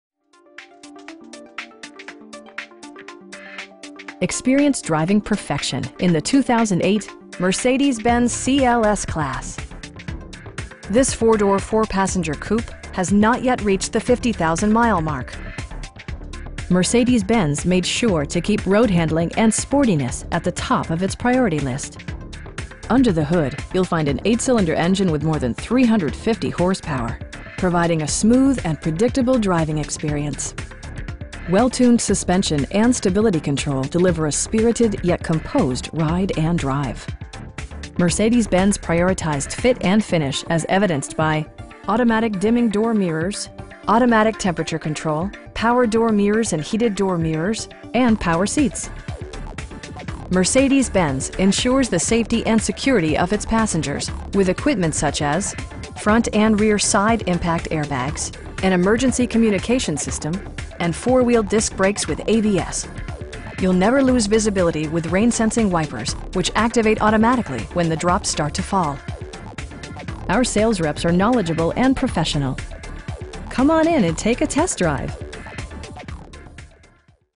2008 Mercedes-Benz CLS-Class CLS550 in Grapevine, TX 76051
Texas Nissan
1401 W State Hwy 114

Experience driving perfection in the 2008 Mercedes-Benz CLS-Class. This 4 door, 4 passenger coupe has not yet reached the 50,000 mile mark! Mercedes-Benz made sure to keep road-handling and sportiness at the top of it's priority list. Under the hood you'll find an 8 cylinder engine with more than 350 horsepower, providing a smooth and predictable driving experience. Well tuned suspension and stability control deliver a spirited, yet composed, ride and drive Mercedes-Benz prioritized fit and finish as evidenced by: leather upholstry, an automatic dimming rear-view mirror, automatic dimming door mirrors, automatic temperature control, power door mirrors and heated door mirrors, remote keyless entry, and power seats. Everything is where it ought to be, from the dashboard controls to the door locks and window controls. For drivers who enjoy the natural environment, a power moonroof allows an infusion of fresh air. Mercedes-Benz ensures the safety and security of its passengers with equipment such as: dual front impact airbags, front and rear SIDE impact airbags, traction control, brake assist, anti-whiplash front head restraints, a security system, an emergency communication system, and 4 wheel disc brakes with ABS. You'll never lose visibility with rain sensing wipers, which activate automatically when the drops start to fall. Our sales reps are knowledgeable and professional. We'd be happy to answer any questions that you may have. Come on in and take a test drive!